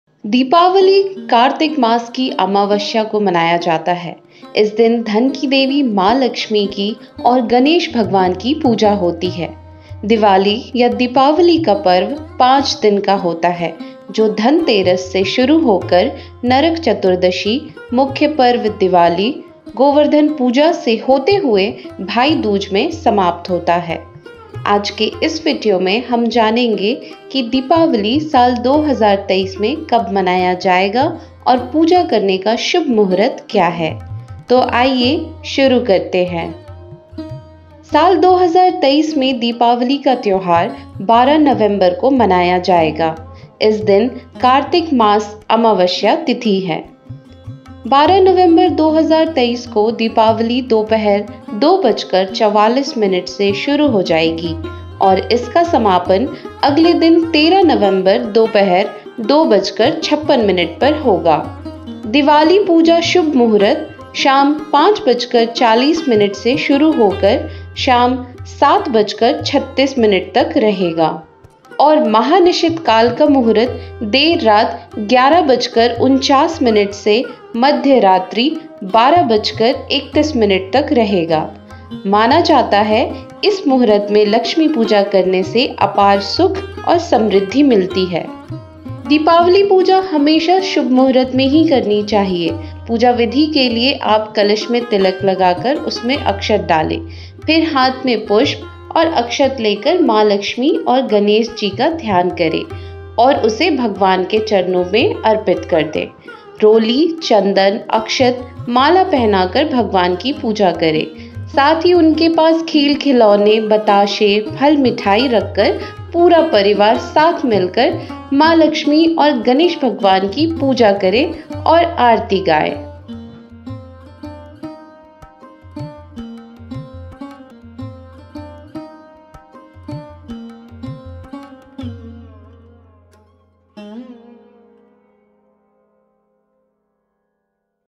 Diwali 2023 Me Kab Hai/Kab Hai Diwali 2023/Diwali Kab Hai 2023/Diwali Puja Shubh Muhurat 2023
दिवाली या दीपावली हर साल कार्तिक मास के अमावस्या को मनाया जाता है. इस साल ये त्यौहार 12 नवम्बर 2023 को मनाया जायेगा। अगर आप लक्समी पूजा शुभ मुहूर्त जानना चाहते है हो इस वीडियो को पूरा देखे।